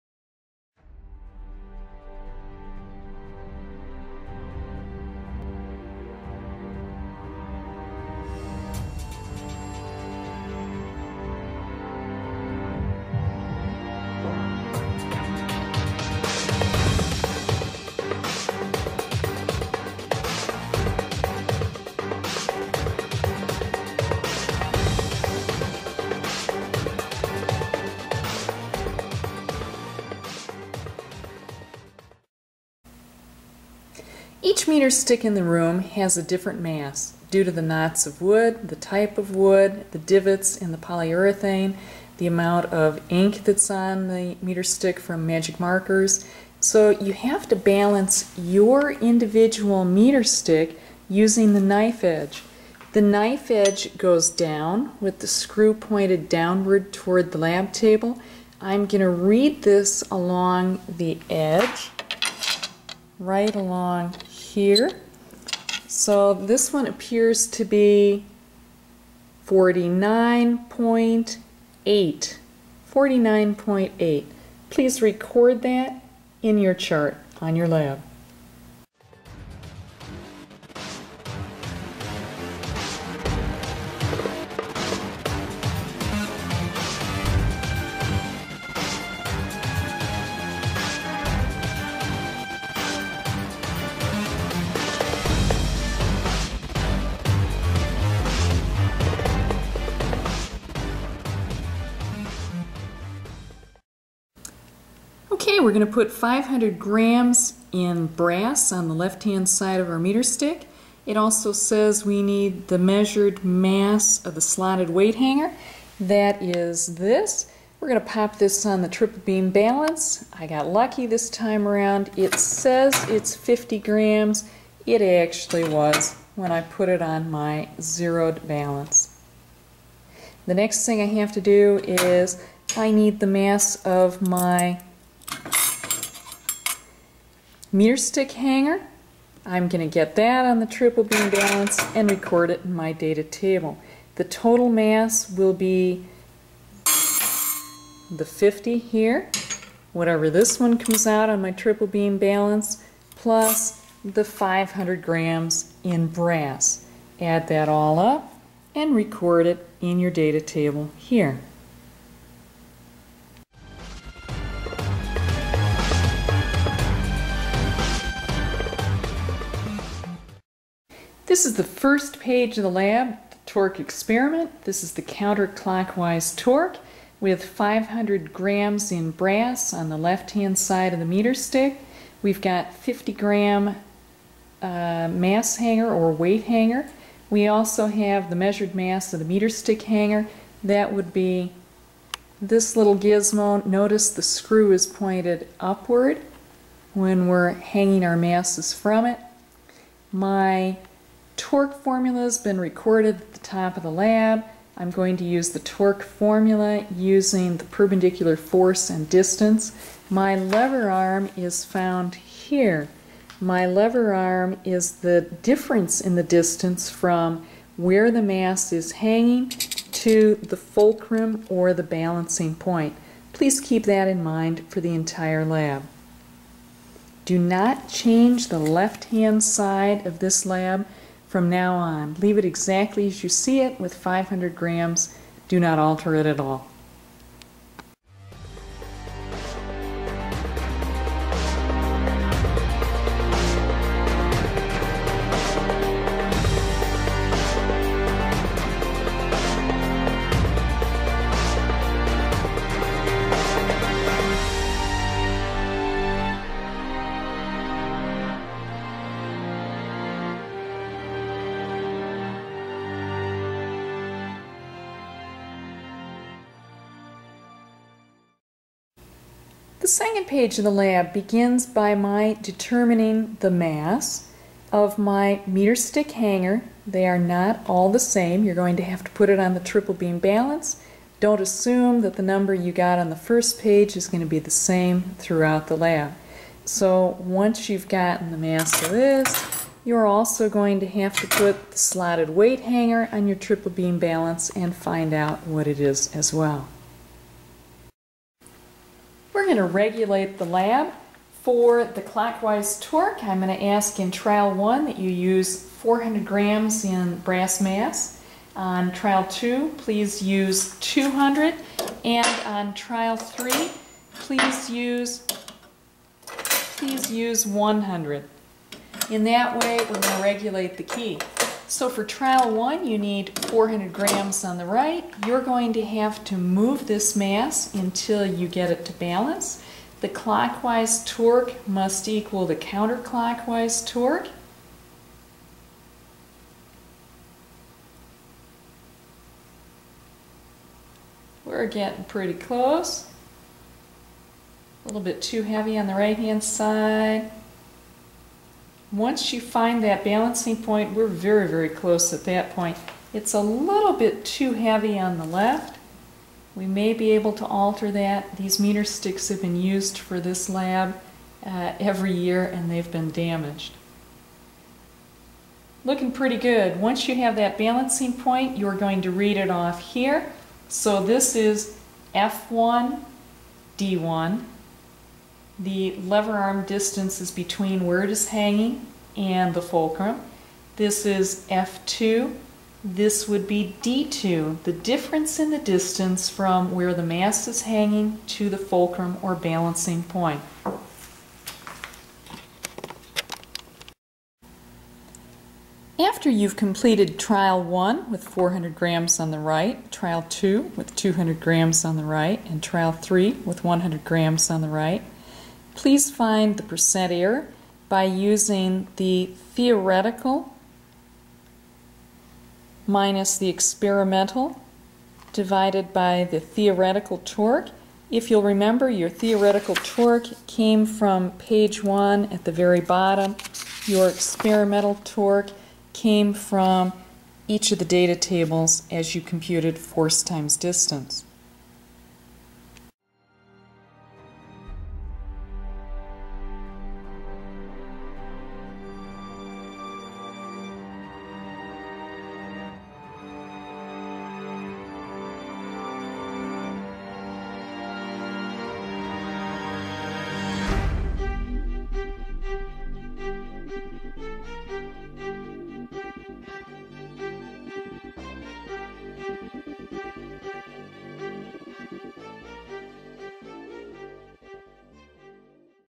Torque Lab 2
Torque is the tendency of a force to produce a rotation around an axis. In this experiment we will examine the clockwise and counterclockwise torque of a meter stick balanced on a fulcrum. This video contains a correction to the percent error requested.